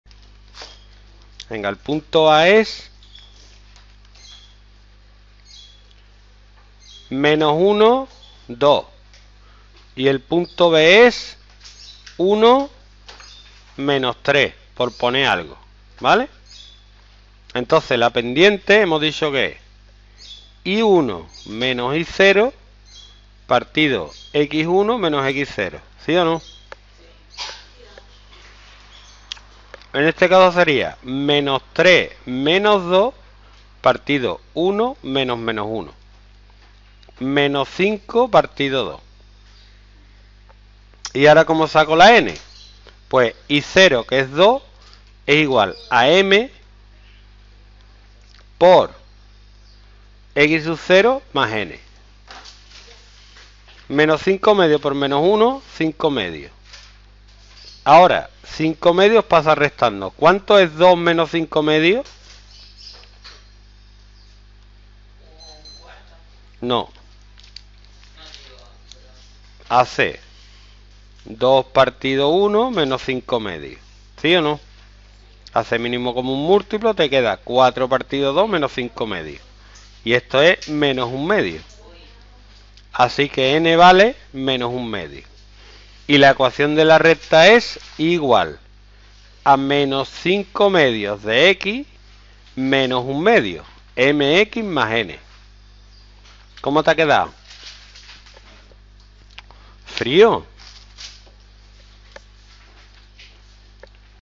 Ecuación de la recta a partir de dos puntos Matemáticas 3º ESO Academia Usero Estepona
La ecuación de la recta se puede calcular sabiendo dos puntos de la misma:
1) Se aplica la fórmula de la pendiente para obtenerla: m=(x1-x0)/(y1-y0)
2) Se sustituyen y0, x0 y n en la ecuación y0=mx0+n y se despeja n.
3) Y sustituyendo los valores de m y n en y=mx+n, se tiene la ecuación de la recta.